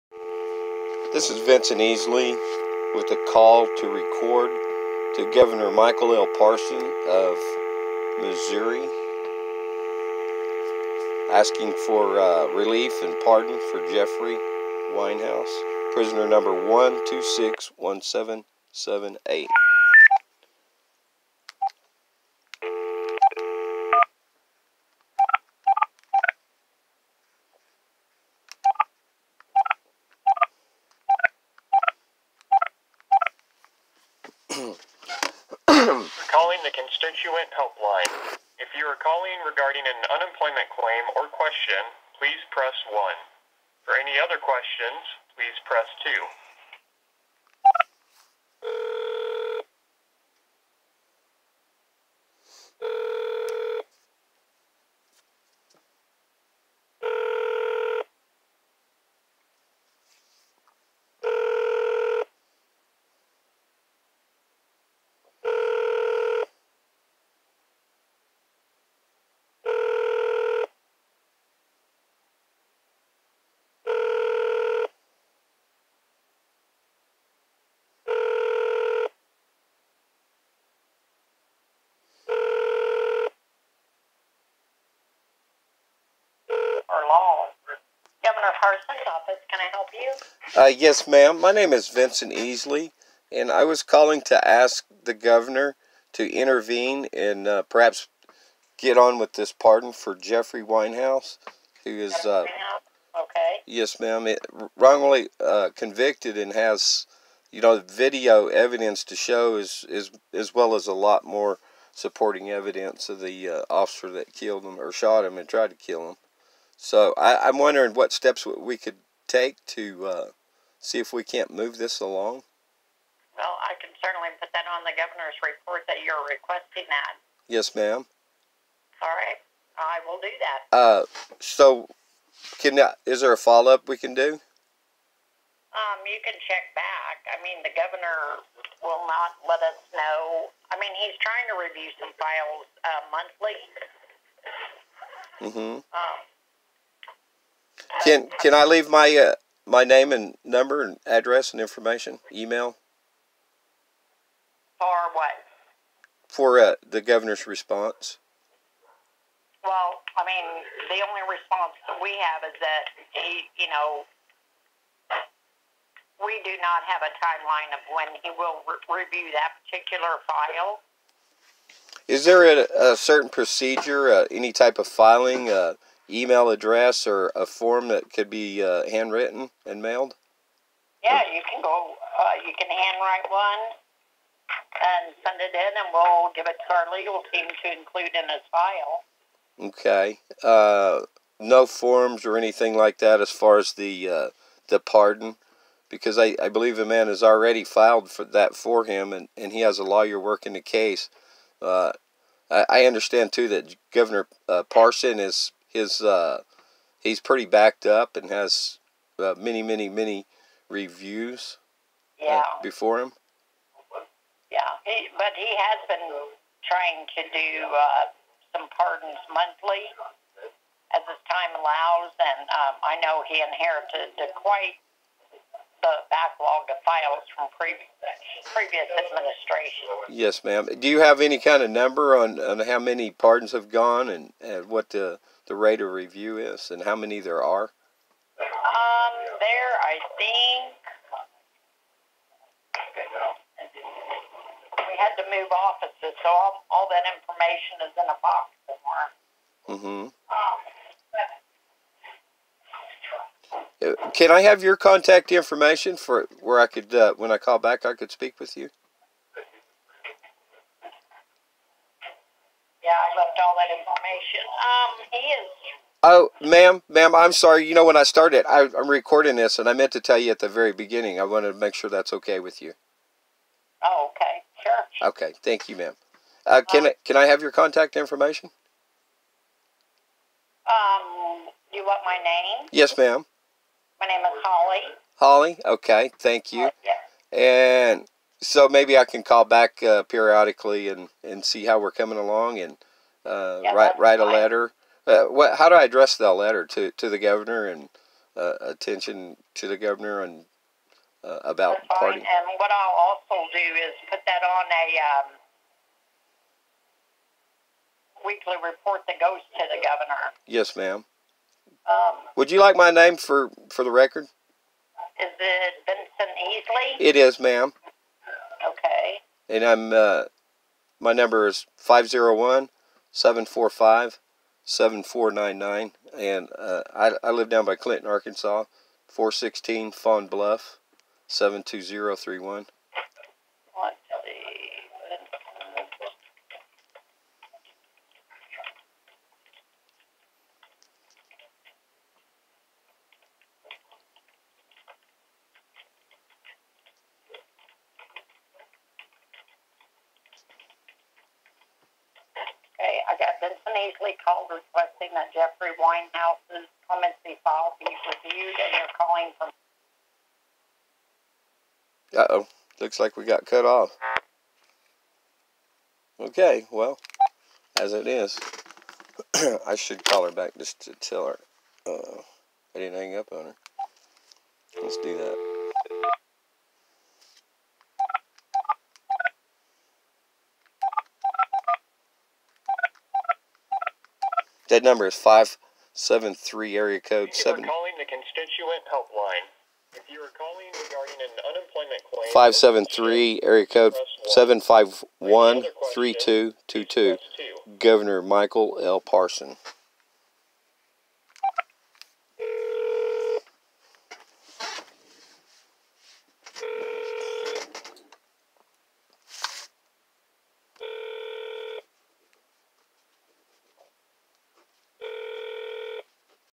Missouri Journalist Silenced, Shot & Caged for Questioning 'Authorities'
This is my call to Missouri Gov. Michael L. Parson's office seeking his review in overturning the wrongful conviction of Jeffrey Weinhaus.

    Jeffrey Weinhaus is INNOCENT!!!
What can you do to help?
Prayers
Educate yourself about Jeffrey's case. Review this EXHAUSTIVE LIST

Visit this website for more including links for the following

Jeffrey Weinhaus Press Release

Visit Jeff Weinhaus page on facebook

Jeffrey Weinhaus Electronic Dropbox

Visit JeffreyWeinhaus.com which replaced FreeJeffNow.com

Write him an encouraging letter

If you are a Christian, join us on Saturday's call

Pass out American Political Prisoner Business Cards

Visit Jeffrey in prison

Read Warren Markowitz Legal Documents

Send email to Jeff via Jpay.com

Read This Article

Note: Jeffrey can NOT receive hardback books, only paperback (3/13/19)

Please spend some time on this webpage to learn about Jeffrey Weinhaus. Ask yourself if this is justice and do you want to live in a country where this is allowed.